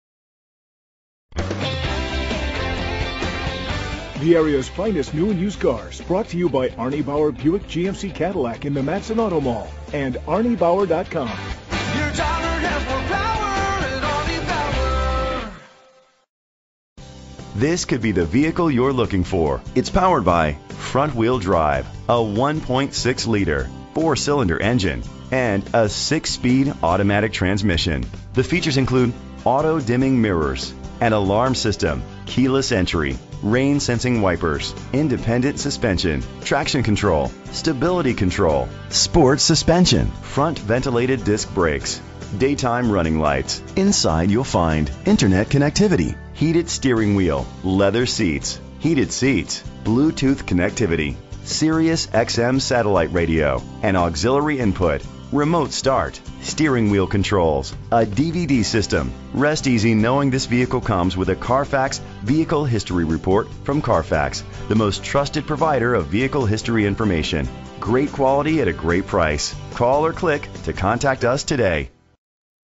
2016 Buick Cascada B62256 - Matteson IL
Year: 2016
Make: Buick
Model: Cascada
Trim: Premium
Engine: 1.6 liter 4 Cylindercylinder FWD
Transmission: 6-Speed Automatic
Color:
Mileage: 0
Address:
5525 Miller Circle Drive
Matteson, IL 60443
VIN:W04WT3N50GG063571